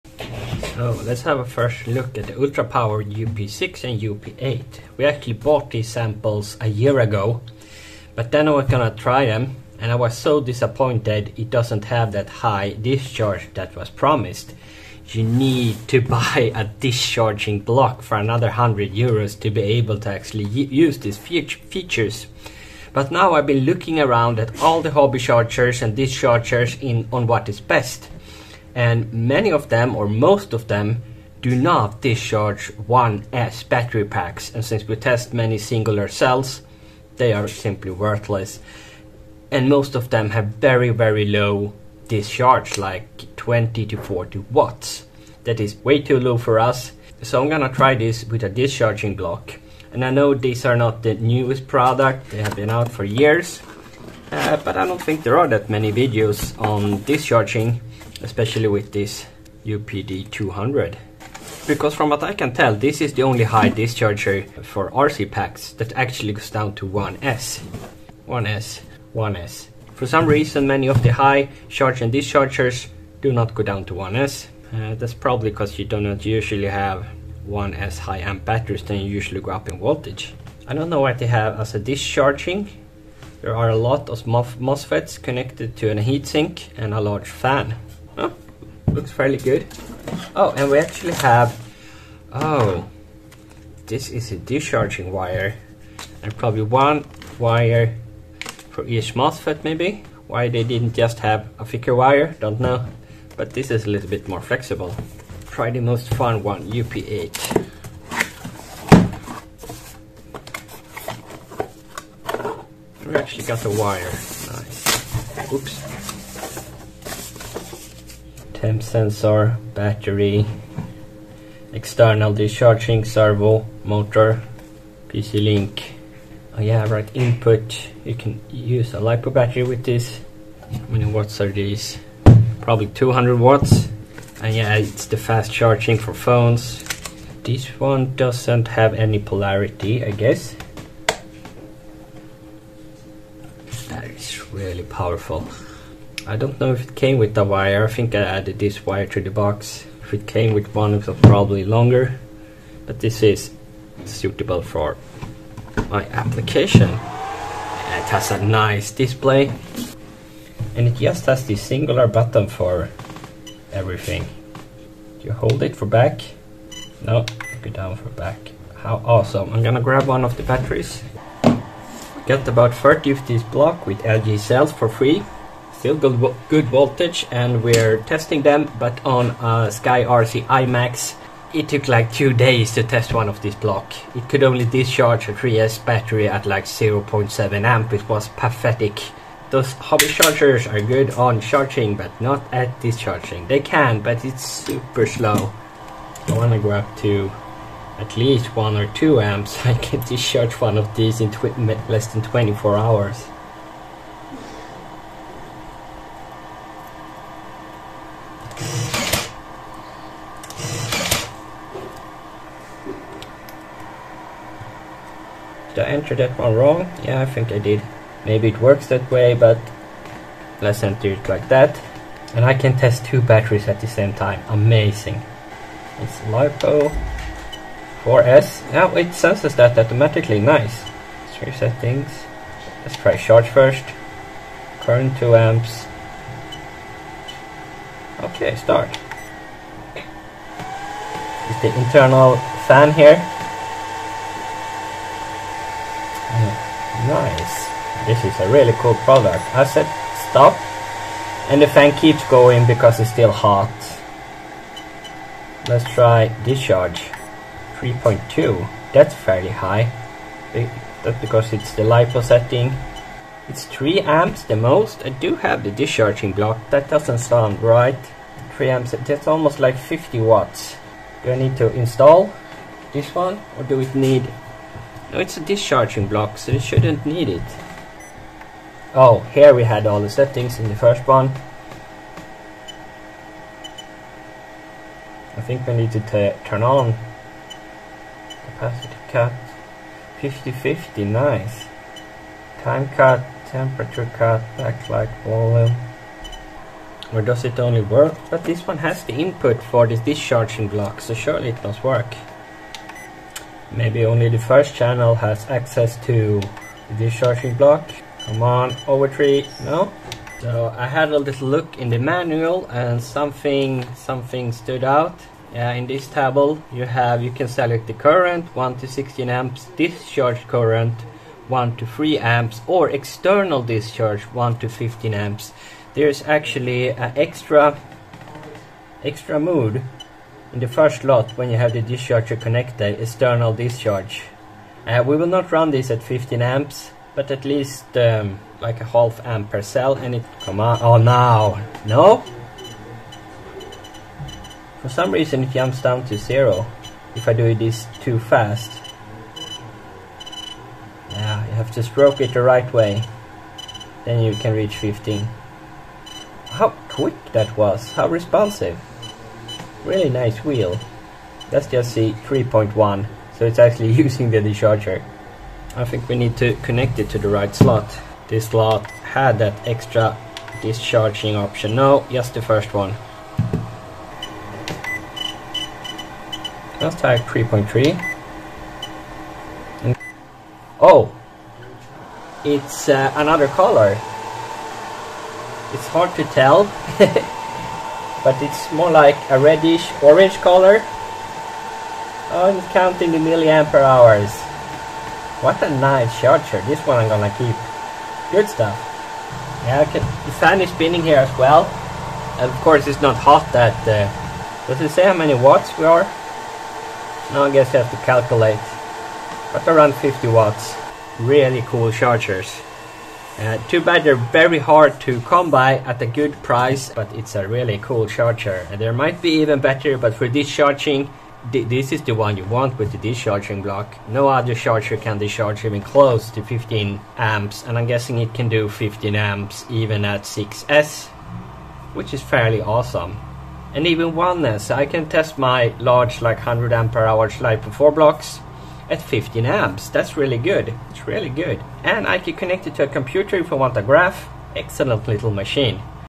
Just playing around a bit with my Ultra Power UP8 and UP-D200 (200W 15A) External Discharger
Probablly the best 1-6S Discharger?